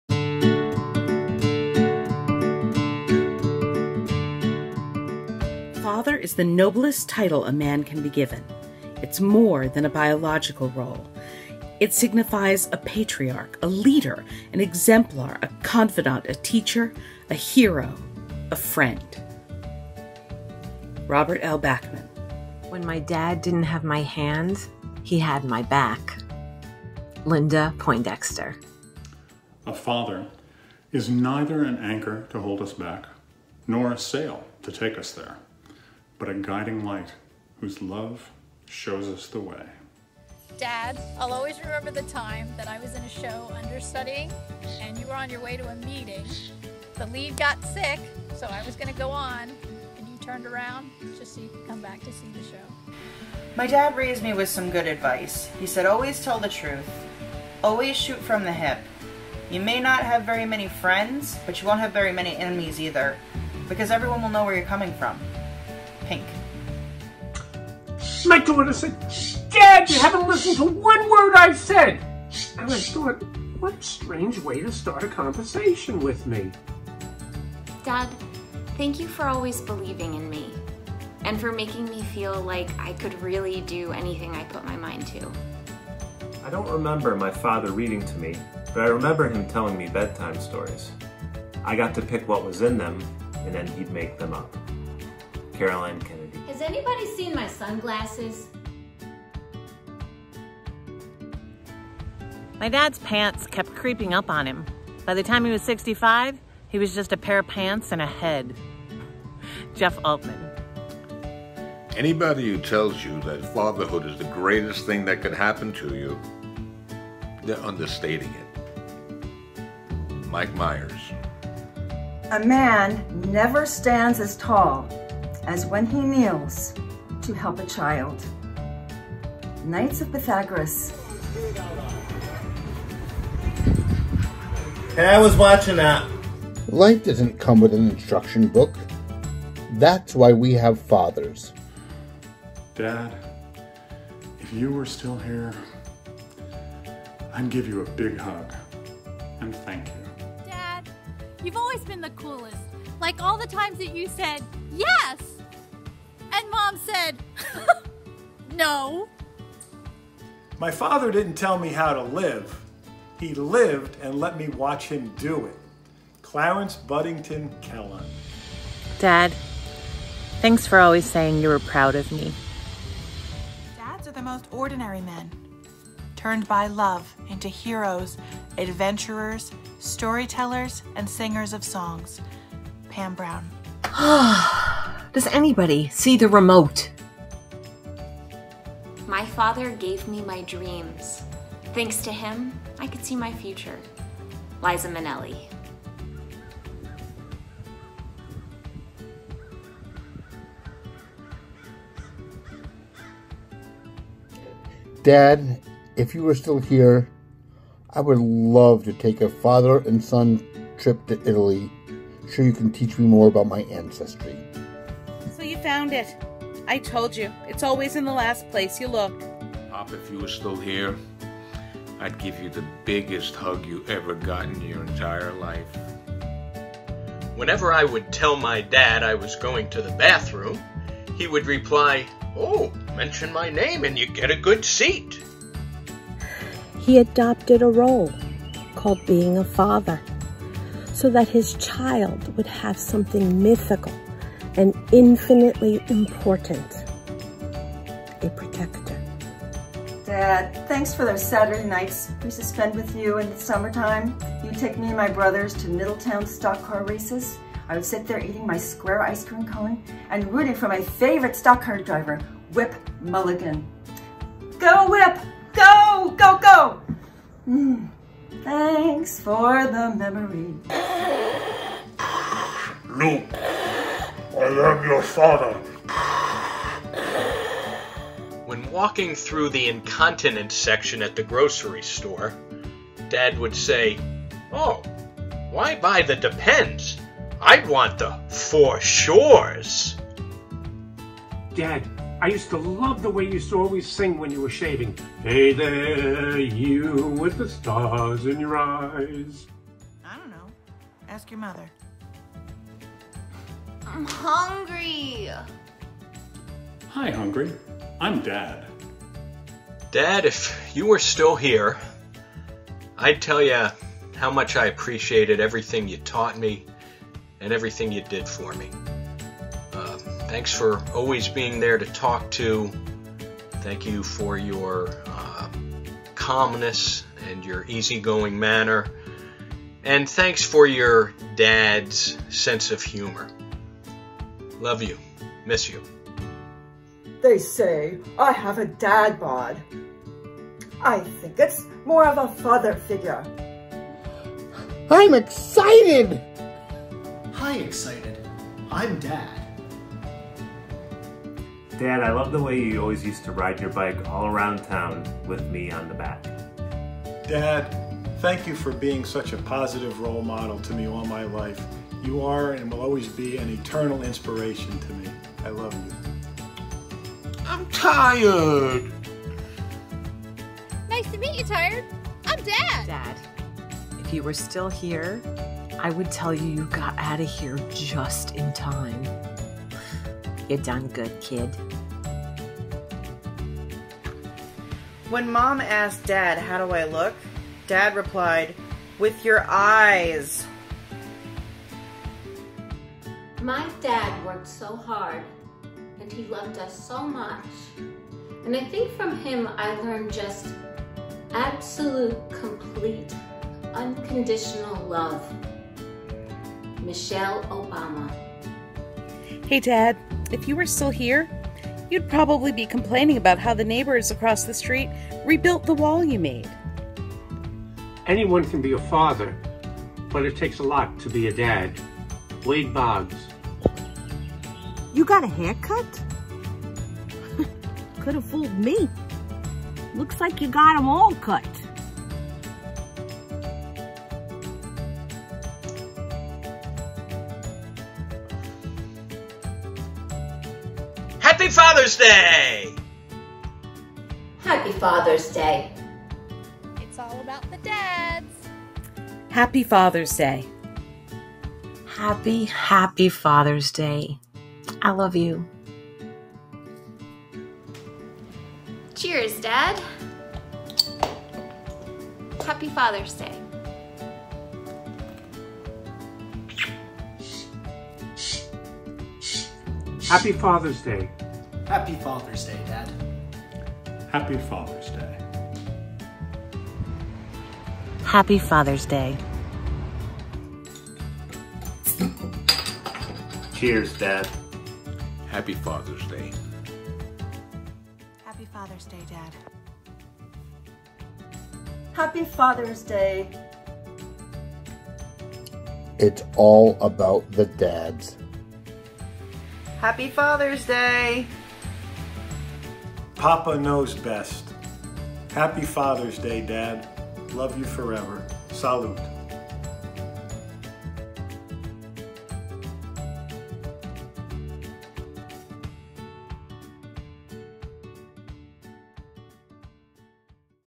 "Papa Knows Best" Happy Father's Day! from BTC
Happy Father's Day from the Brewster Theater Company!

We are pleased to present "Papa Knows Best!", celebrating all of the fathers in our lives! Created and edited by Jenna Isabella, featuring Tamara Alexander, Judy Brewster, Nate Brewster, Janette Devine, Anna Fazio, Jenna Isabella, John Isabella III, John Isabella Jr, Barbara Kessler, Michelle Marotz, Kevin McCarthy, Robin O'Leary, Frank Petruccelli, Ruby Rocco, Ted Schwartz, Ken Shelby, and Gina Stanton.